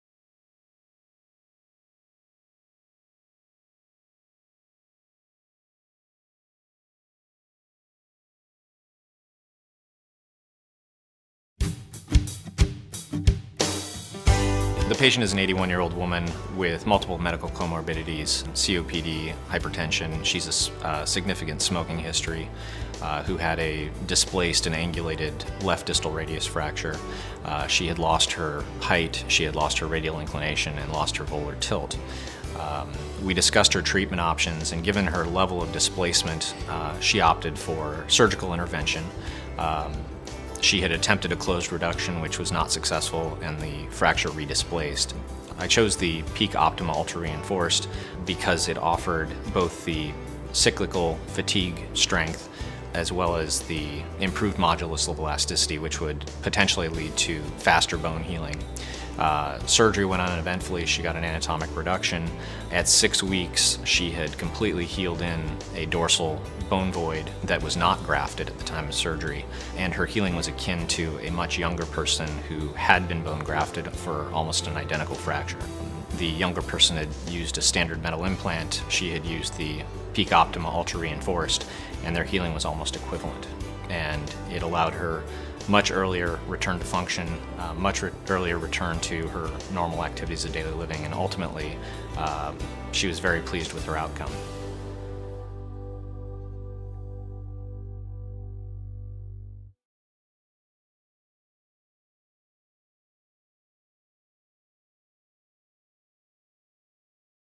Case Study: Distal Radius Fracture
Hear Dr. Adam Rives, M.D. describe the outcomes of an elderly smoker patient who was implanted with a distal radius plate composed of PEEK-OPTIMA Ultra-Reinforced.